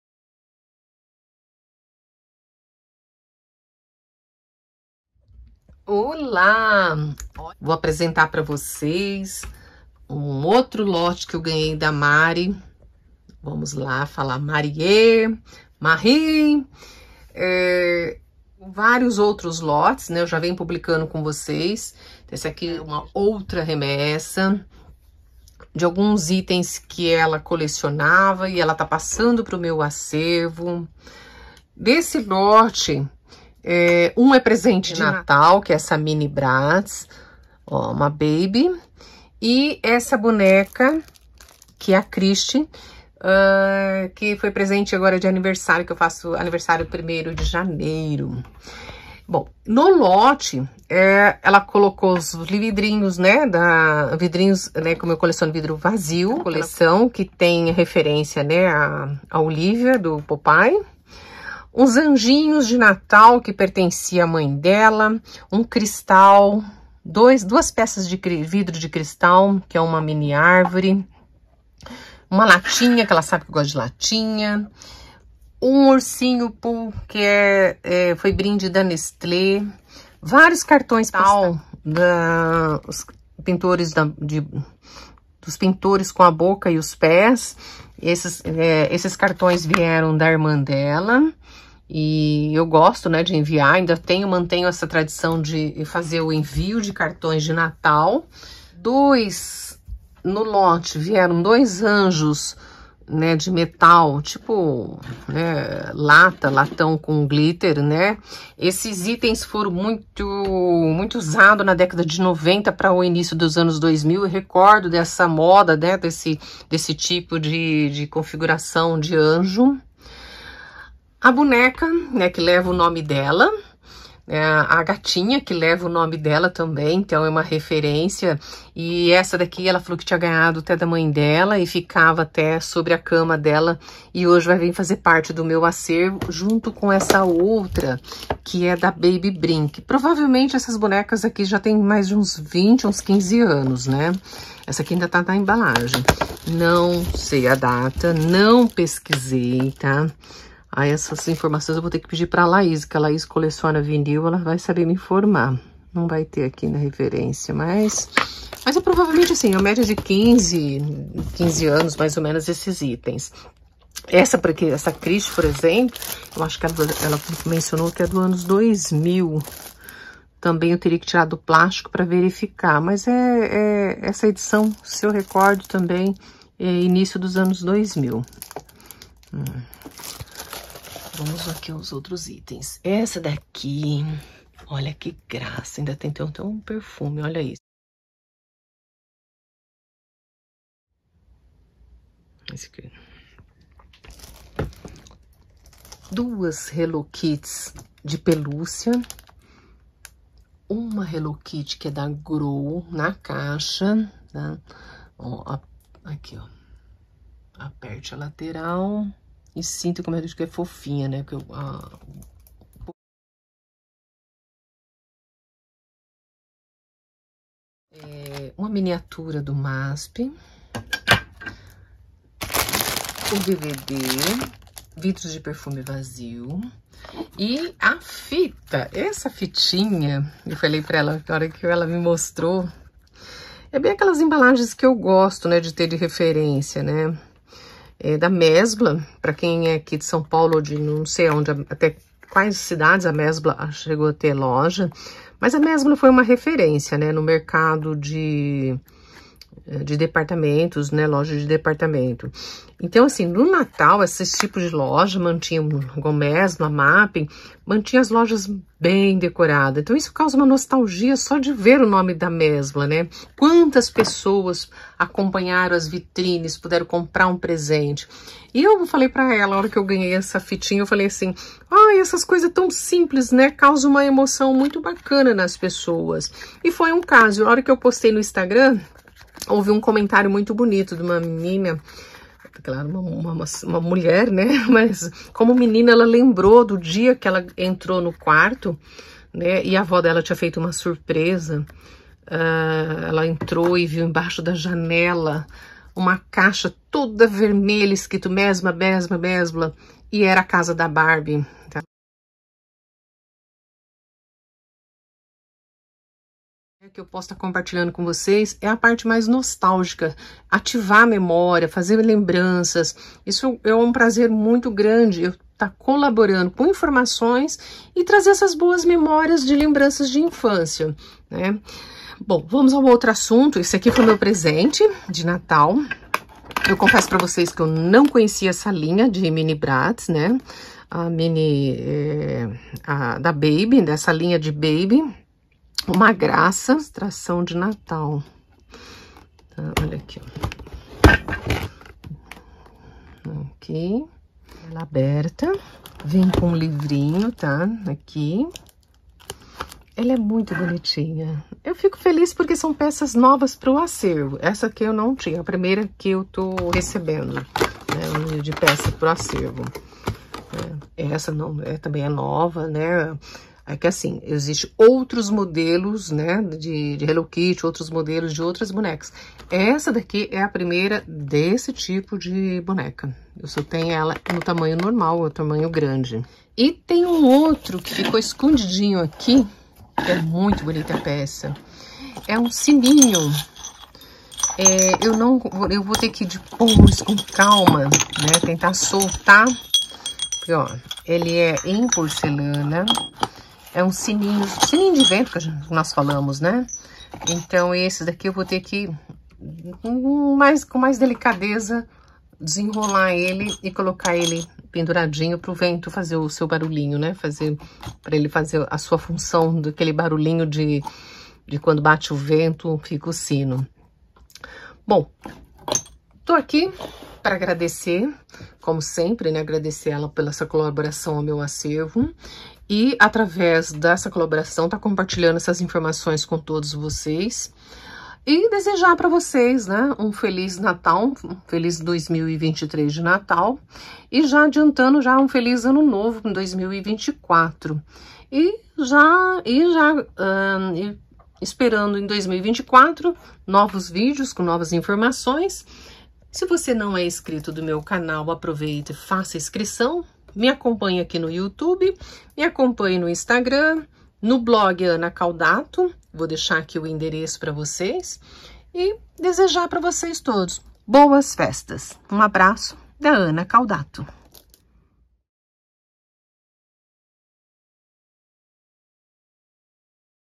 11° Lote Doação ao Acervo Ana Caldatto por Marie
Vídeo apresento o 11° Lote doado pela da Marie ao meu acervo ana caldatto com itens em torno de 20 anos, ou geração início ano 2000 de brinquedos.
Desse lote dois em específico são o meu presente de natal com a mini boneca Bratz da linha distribuída no Brasil pela Gulliver a Bratz Babyz e de Presente aniversário ganhei a boneca Barbie Christie Califórnia 😻